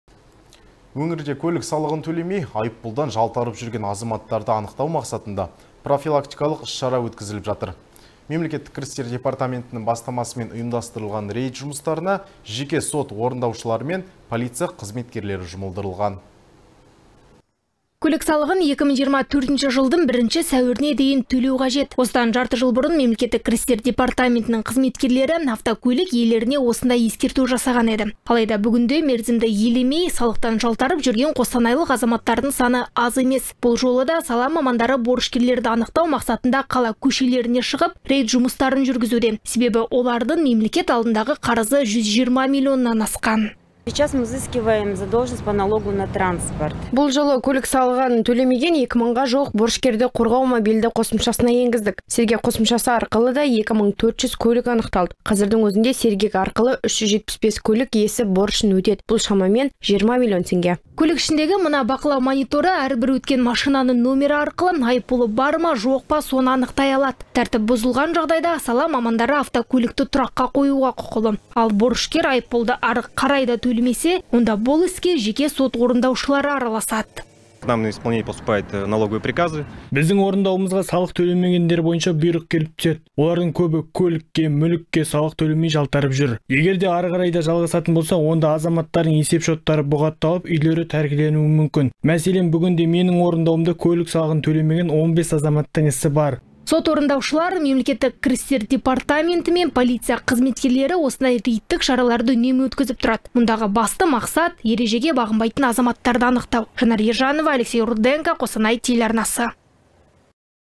Көлік салығы бойынша рейд жүргізілді
Өңірде көлік салығын төлемей, айыппұлдан жалтарып жүрген азаматтарды анықтау мақсатында профилактикалық іс-шара өткізіп жатыр. Мемлекеттік кірістер департаментінің бастамасымен ұйымдастырылған рейд жұмыстарына жеке сот орындаушылары мен полиция қызметкерлері жұмылдырылған.